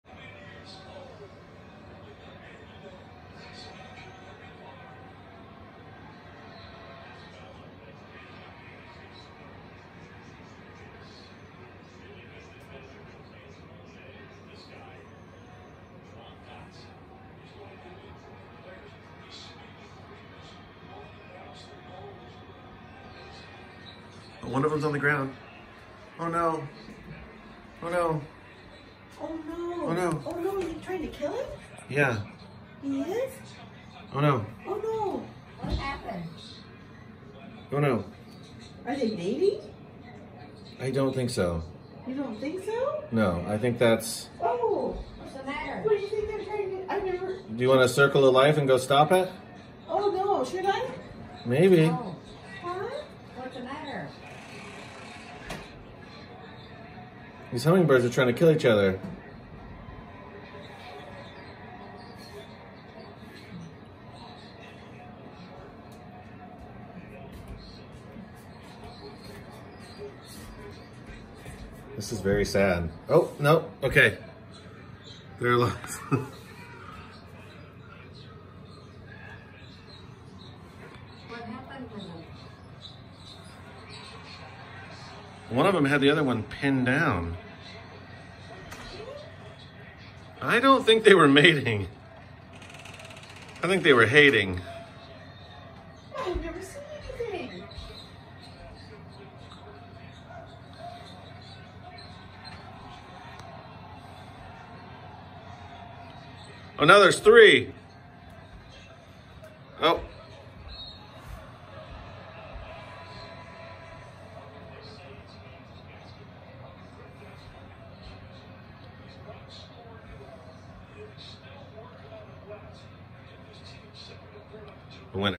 Hummingbird Domination Fight
Luckily it wasn’t a fight to the death.